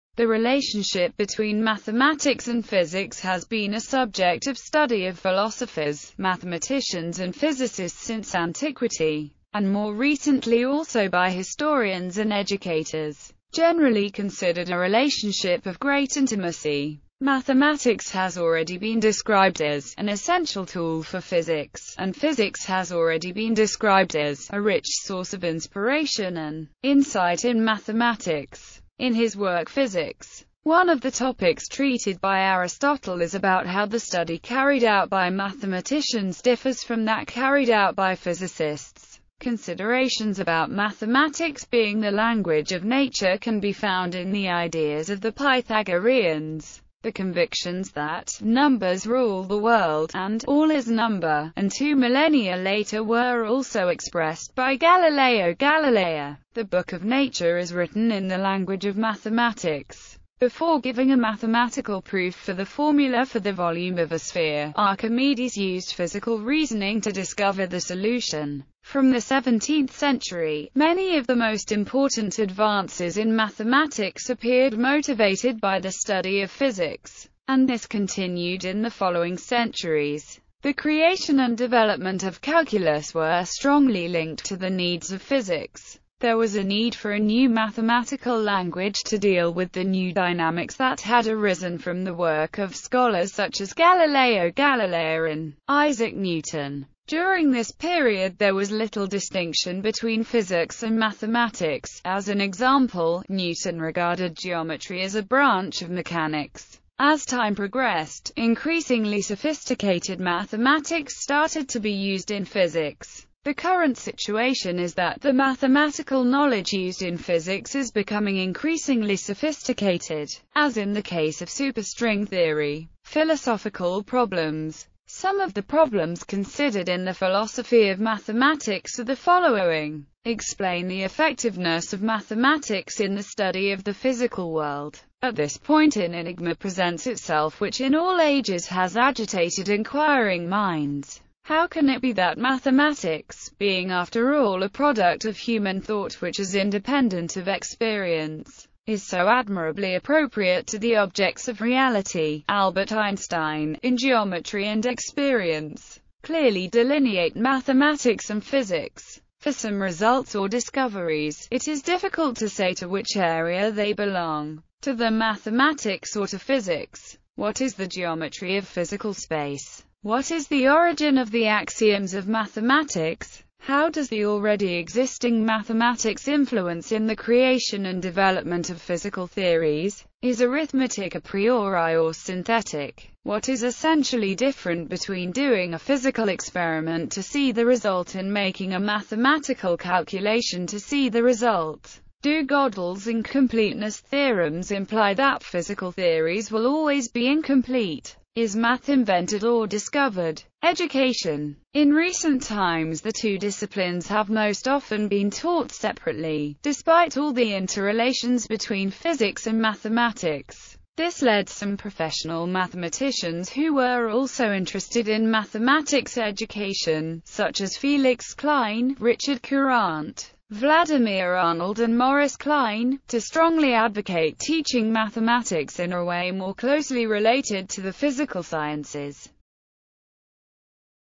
Relationship between mathematics and physics
The relationship between mathematics and physics has been a subject of study of philosophers, mathematicians and physicists since Antiquity, and more recently also by historians and educators.Generally considered a relationship of great intimacy, mathematics has already been described as "an essential tool for physics" and physics has already been described as "a rich source of inspiration and insight in mathematics".